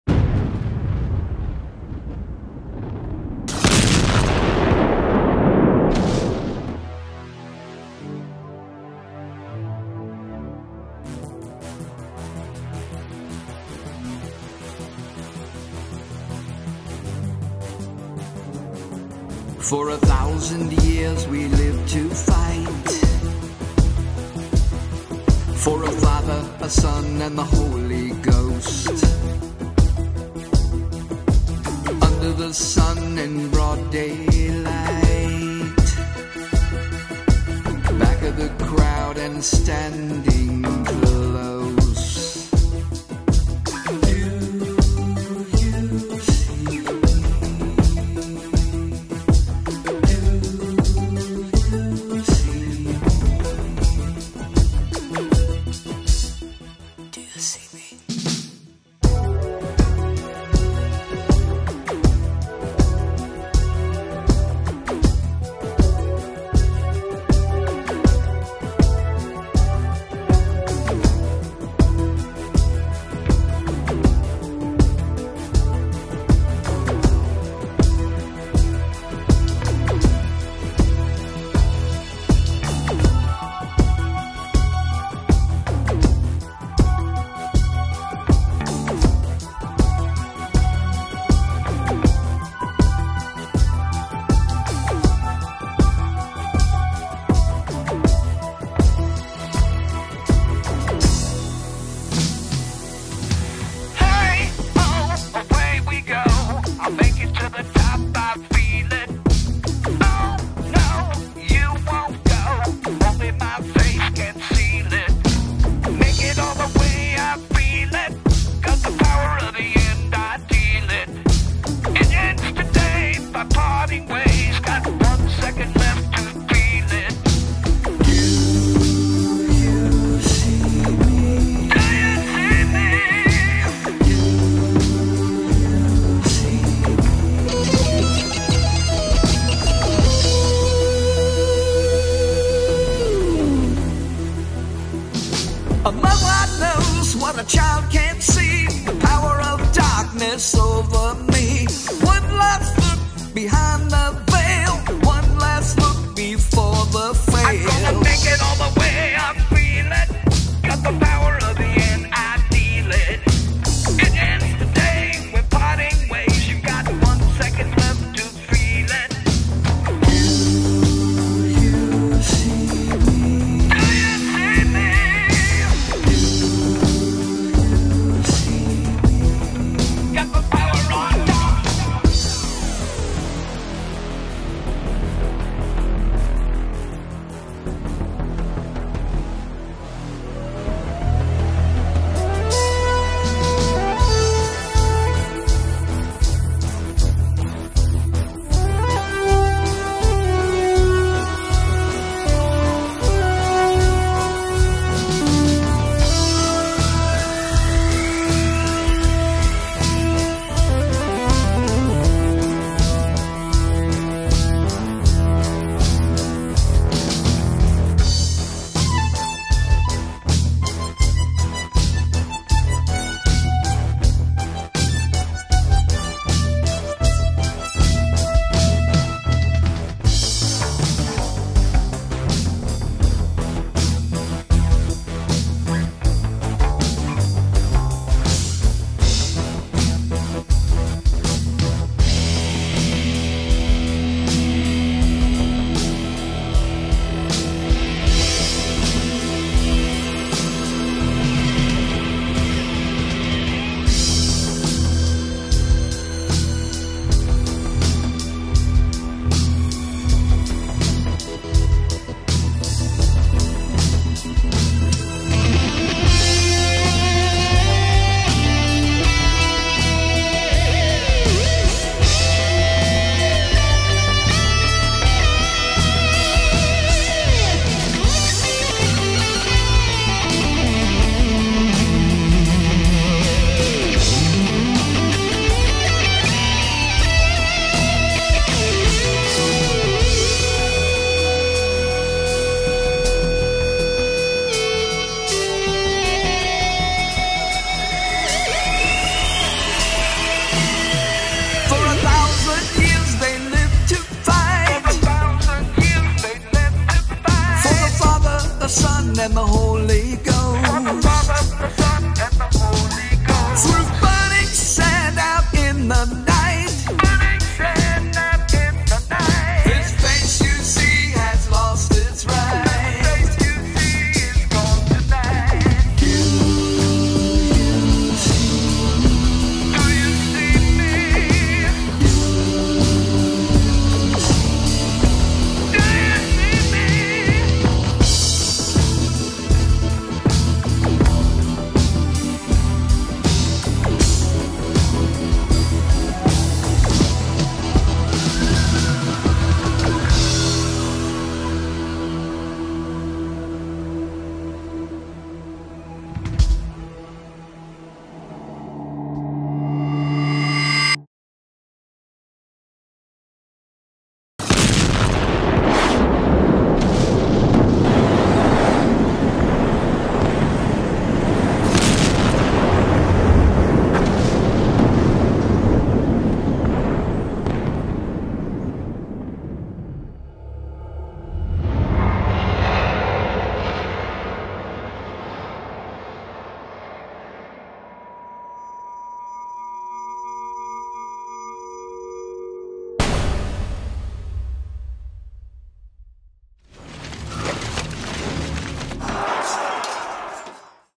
ONE SECOND LEFT - Original Music w/ FRIGHTENING AI Generated Story Video
That moment when she see's she has ONE SECOND LEFT of life...